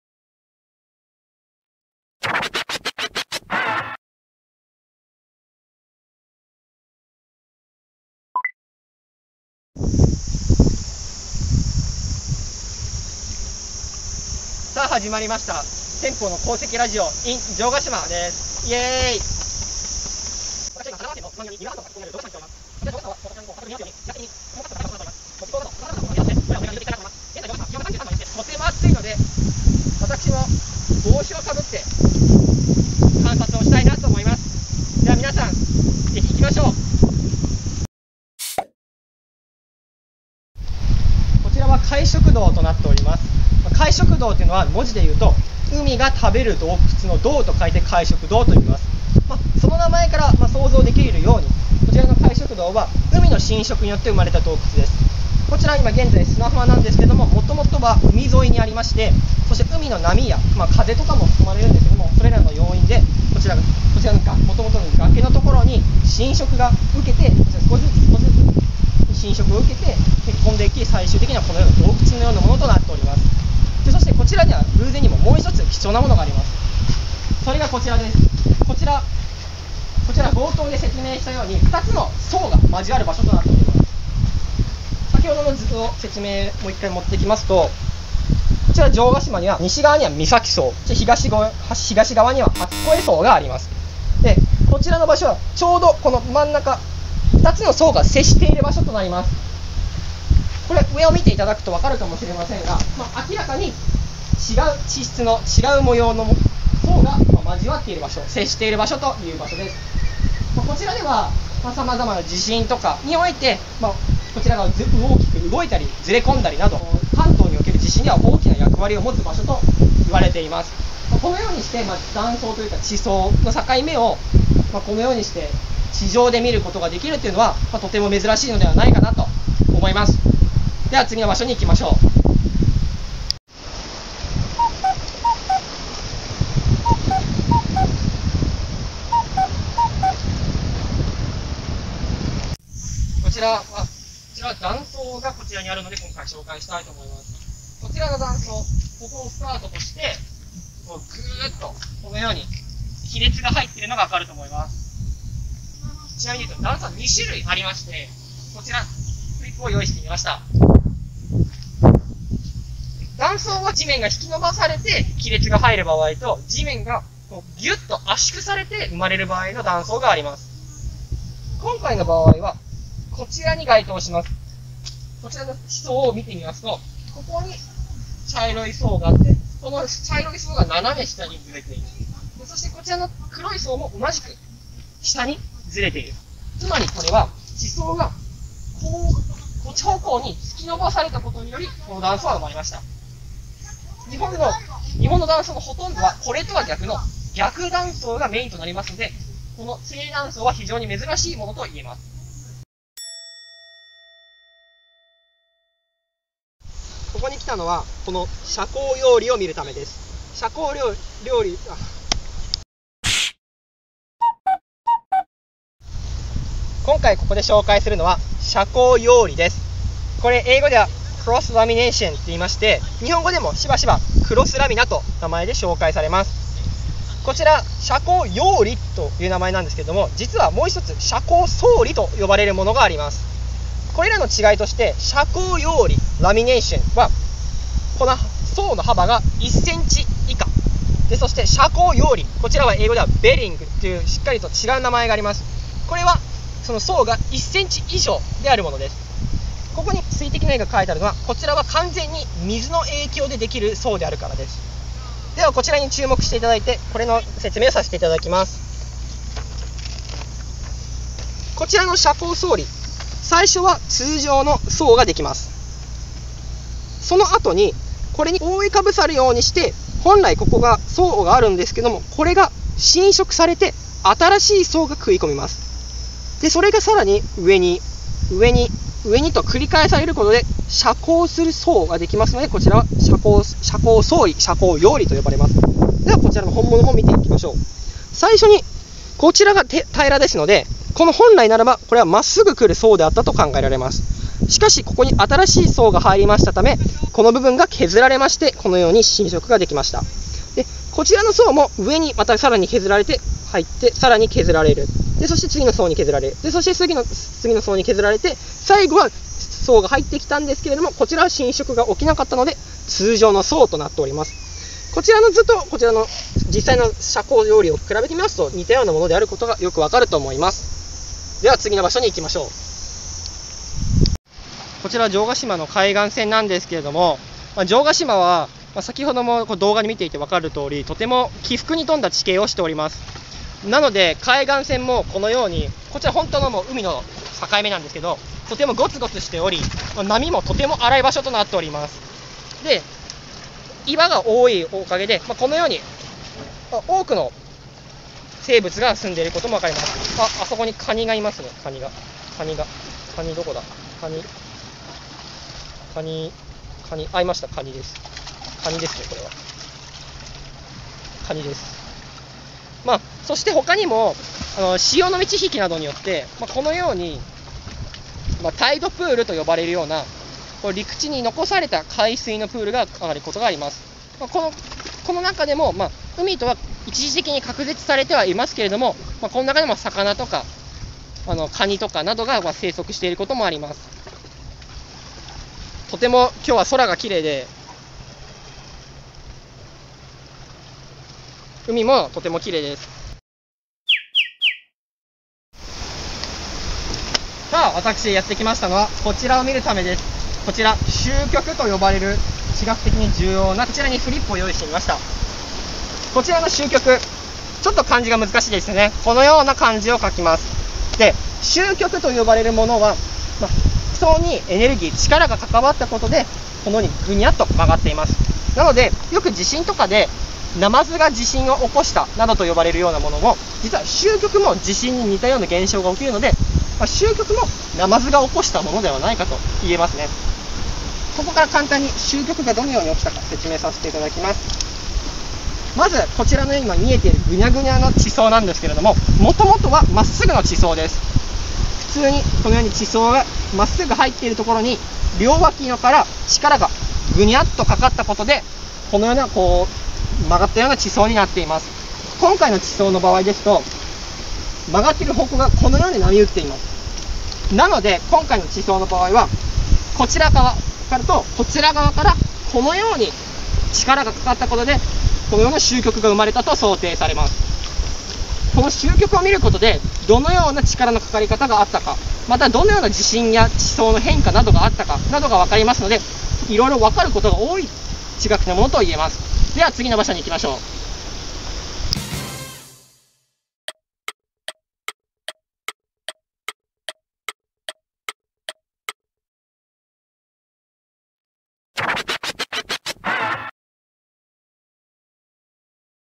地学好きの大学生が「城ヶ島」を楽しんだだけ②
鉱物採集家せんぽーです。
今回は城ヶ島に行って、地学を満喫してきました。
この動画はその第２回です。

鉱物をメインに日常の地学を更新中！